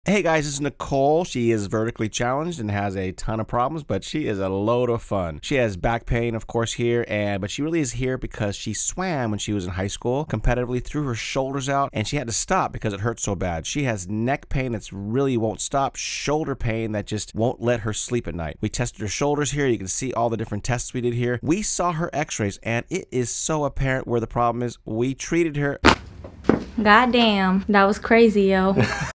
18 y/o Sleep Issues, Neck Pain, Shoulder Pain gets Cracking
18 year old Nicole was a competitive swimming in High School and hurt her neck and shoulders so bad that she had to stop. Her father is a patient in this off and decided to bring her in. We spent 2 week with her before she ran off to college. She not only had neck and shoulder pain but she has years of sleep issues. Check out her story. What a fun girl that made for a fun video. We went all over the place but I think you will still appreciate the story. Please watch his story and see what can be achieved in just a couple of days.